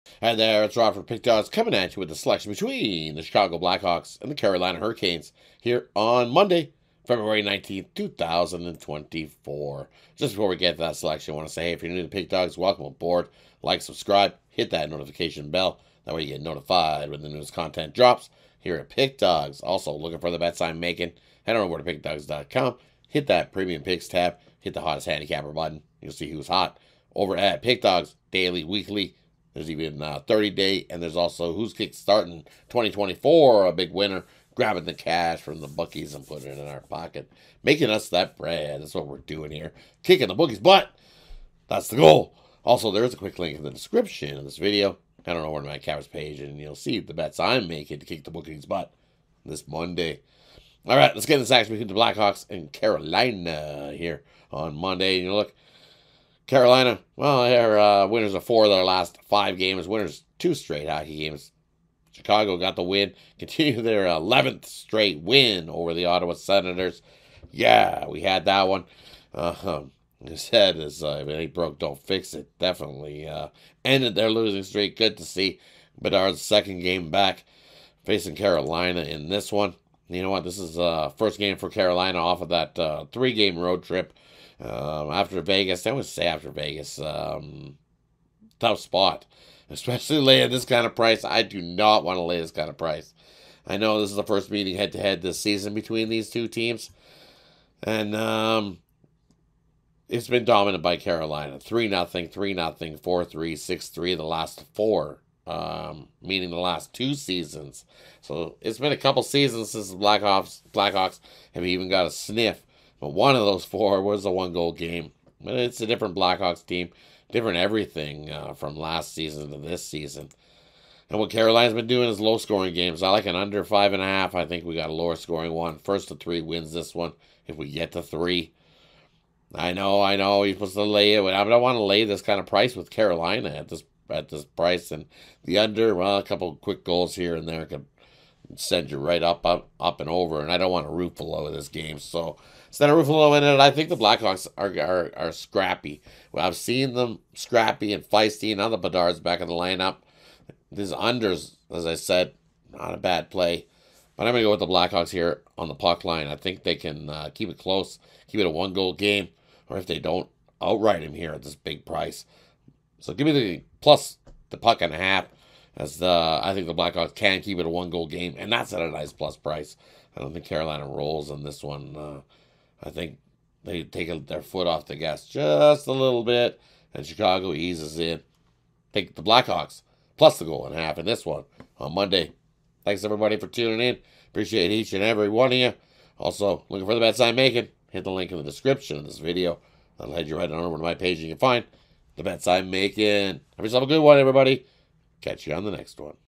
Carolina Hurricanes vs Chicago Blackhawks 2/19/24 NHL Free Pick | NHL Betting Tips
The Chicago Blackhawks face the Carolina Hurricanes in Monday's NHL action. Get Rodd's NHL Pick and Prediction for this game.